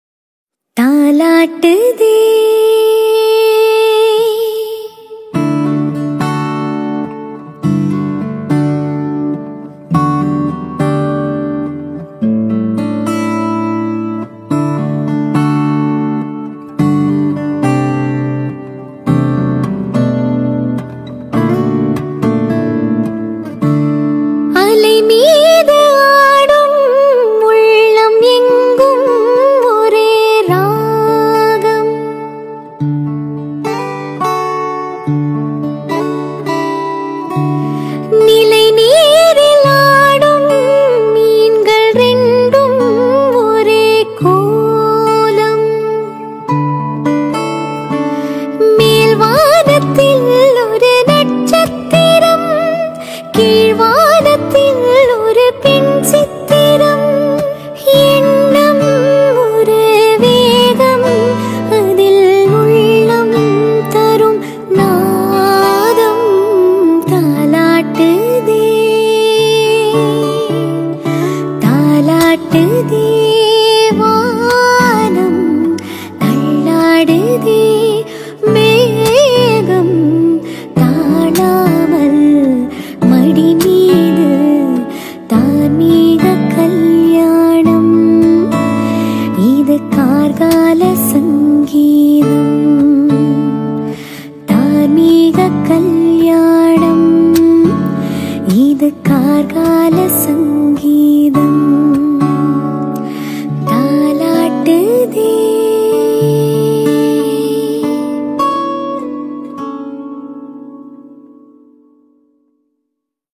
Tamil music lovers 2
Beautiful voice of Saindhavi and beautifully played guitar